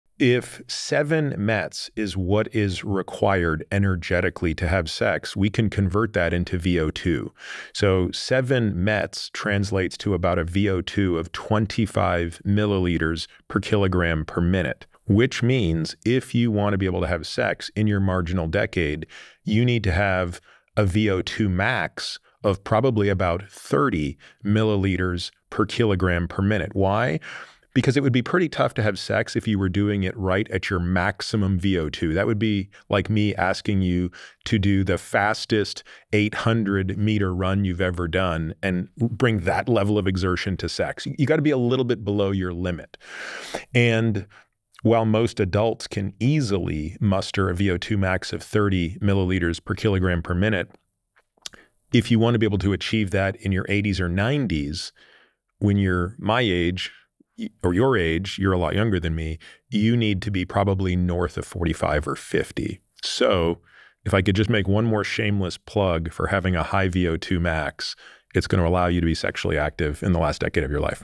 The VO₂ Max You Need to Stay Sexually Active | Sally Greenwald, M.D.
This clip is from episode #371 ‒ Women’s sexual health: understanding desire, arousal, and orgasms, navigating perimenopause with contraceptives and hormone therapy, enhancing sexual comfort and satisfaction, and more | Sally Greenwald, M.D., M.P.H.

In this clip, they discuss:

- Energy Cost of Sex – Sexual activity is estimated to require about seven METs of effort
- Converting METs to VO₂ – Seven METs corresponds to a VO₂ of roughly 25 ml/kg/min
- Fitness as a Predictor of Late-Life Sexual Activity – Sustaining sexual function late in life depends partly on maintaining sufficient aerobic reserve capacity
- VO₂ Max as a Lifelong Investment – A higher VO₂ max today increases the likelihood of remaining sexually active in the final decade of life